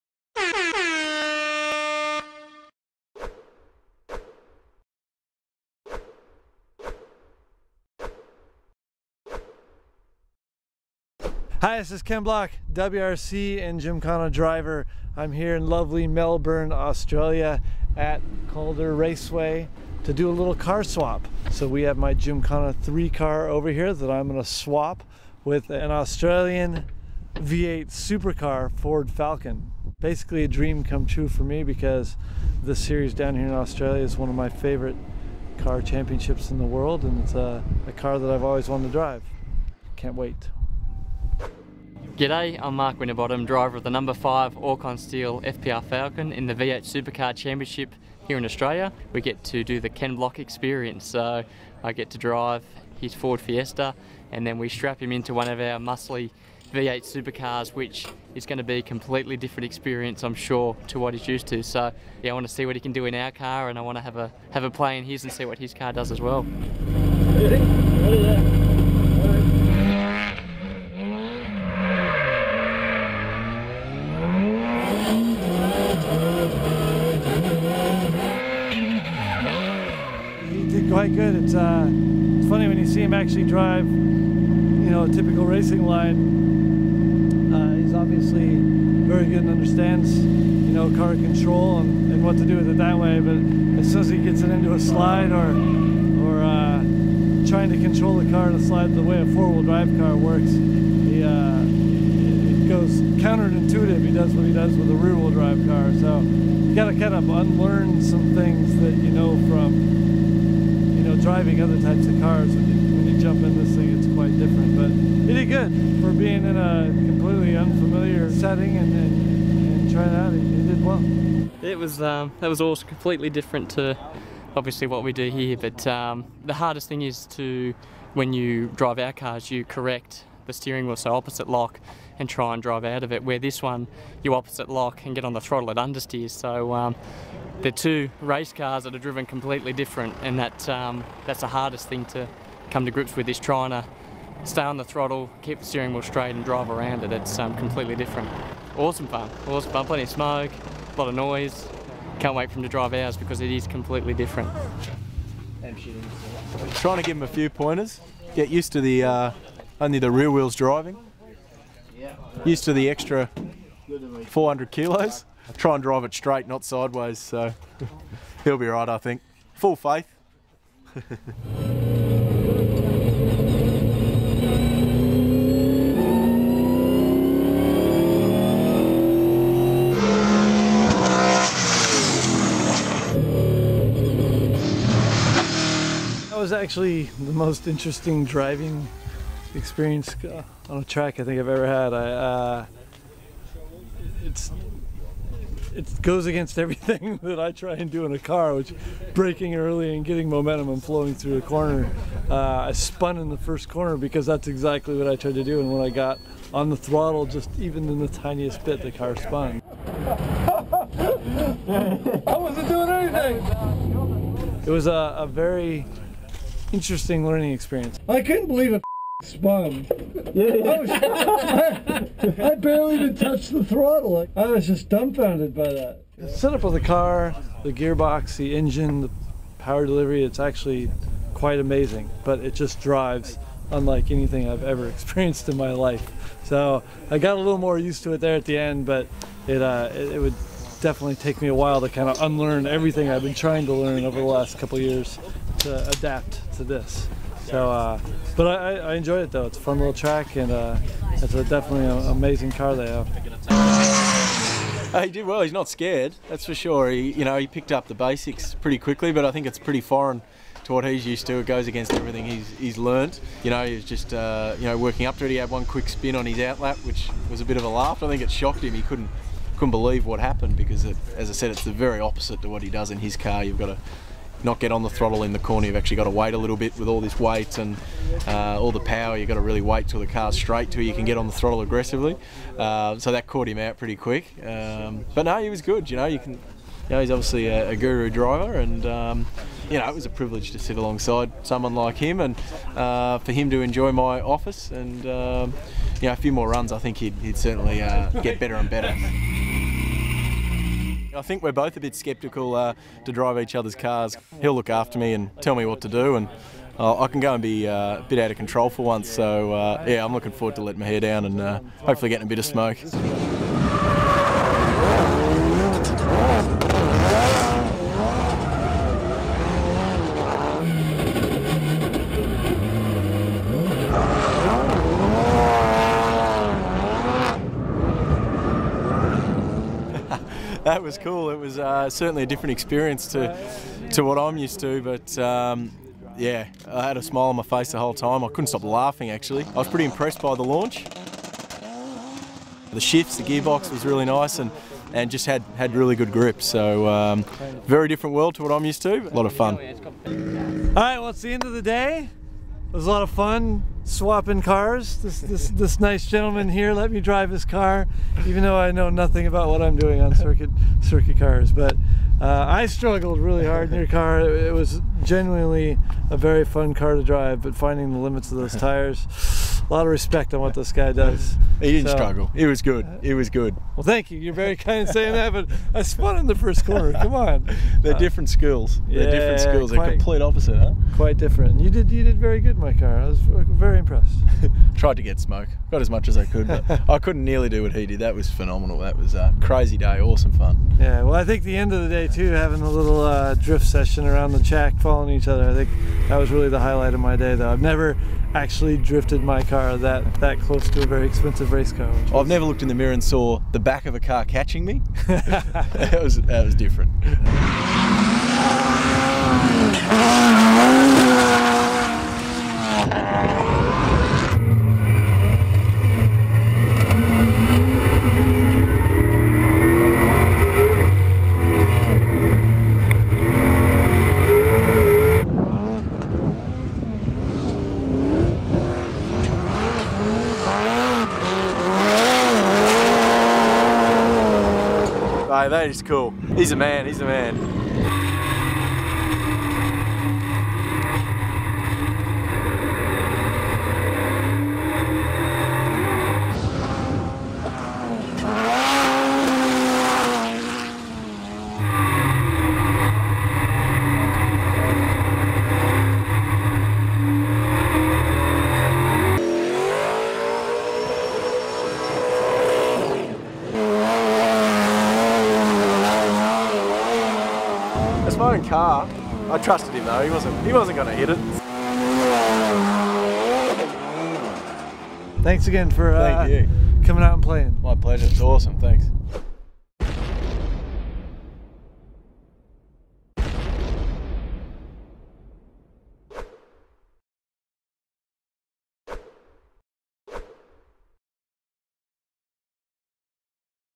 Ken Block swaps seats with V8 Supercar drivers Mark Winterbottom and Will Davison
Ken Block gets behind the (right side) driver's seat of the Ford Falcon V8 Supercar belonging to Australian drivers Mark Winterbottom and Will Davison, while they drive his Gymkhana THREE Ford Fiesta at Calder Park Raceway.